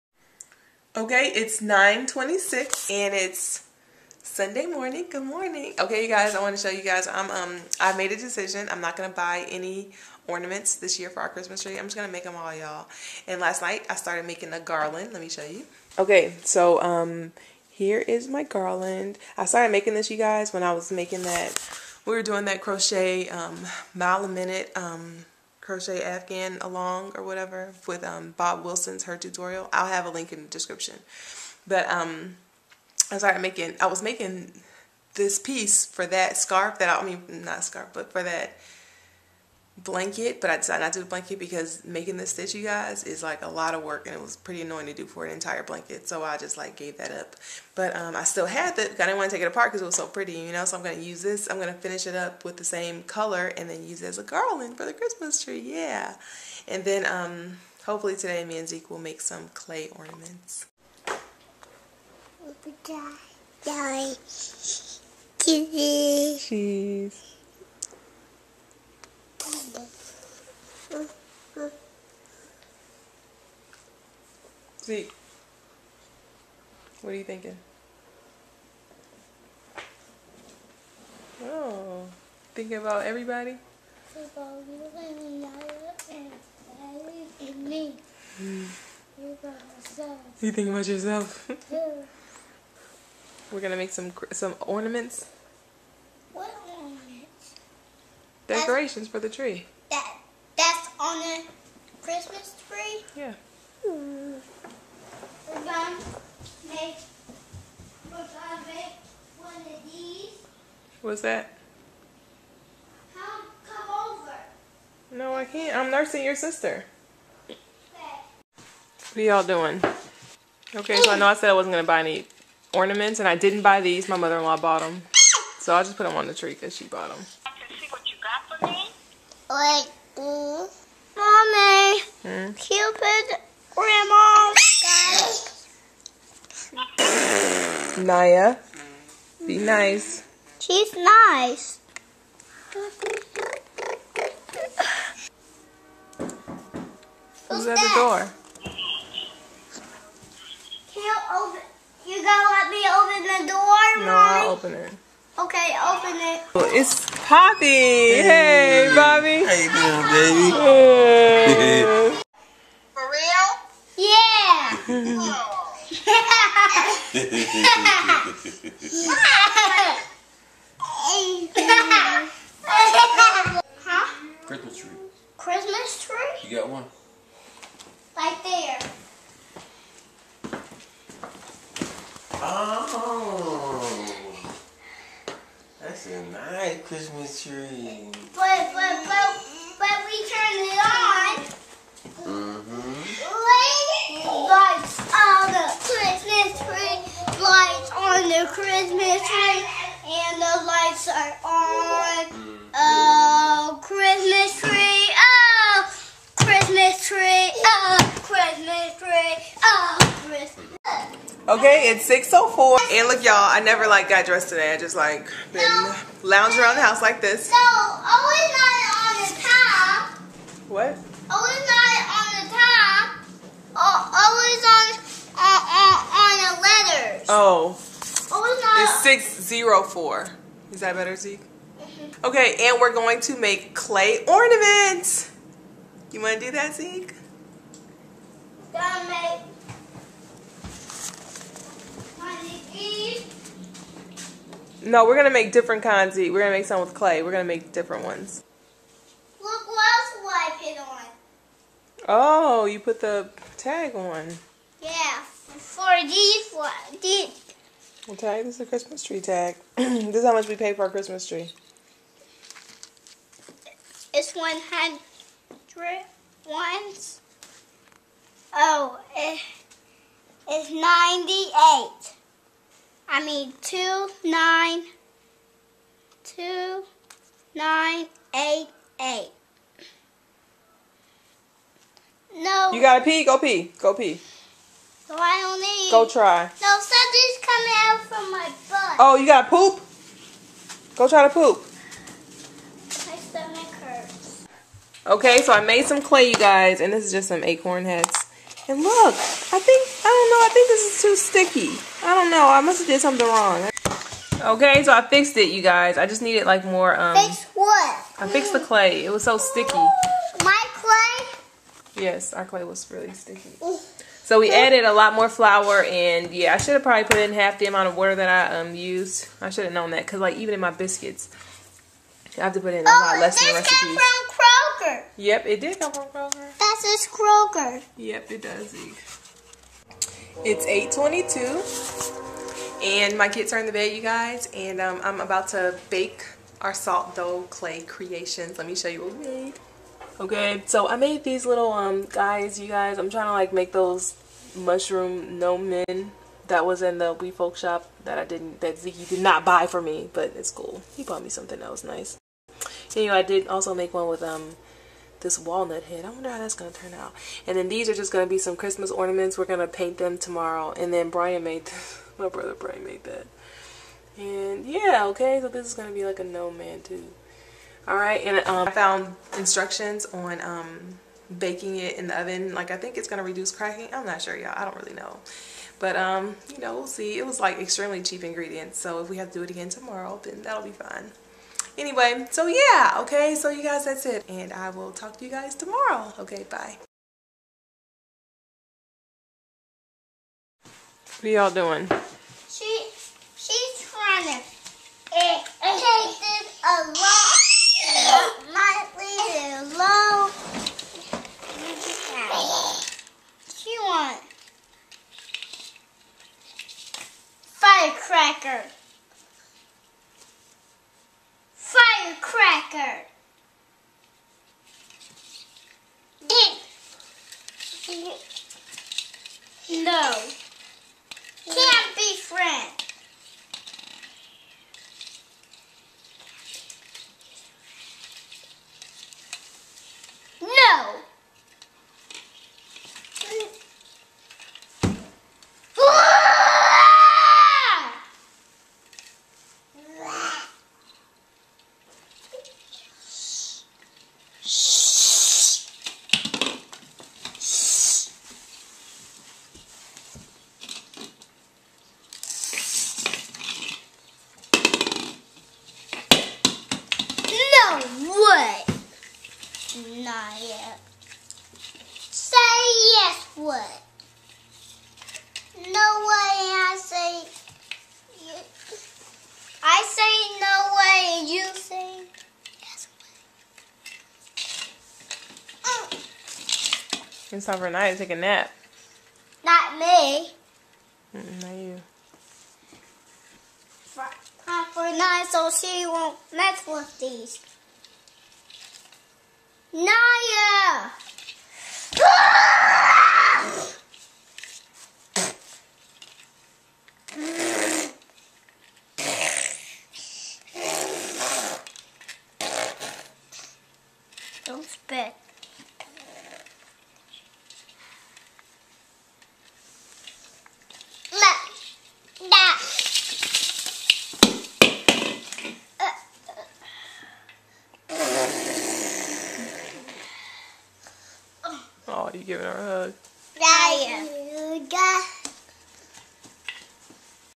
Making Christmas Ornaments from Salt Dough Clay December 9, 2012
I am also working on a garland from this tutorial:
We will paint them tomorrow!  Also, my dad came over.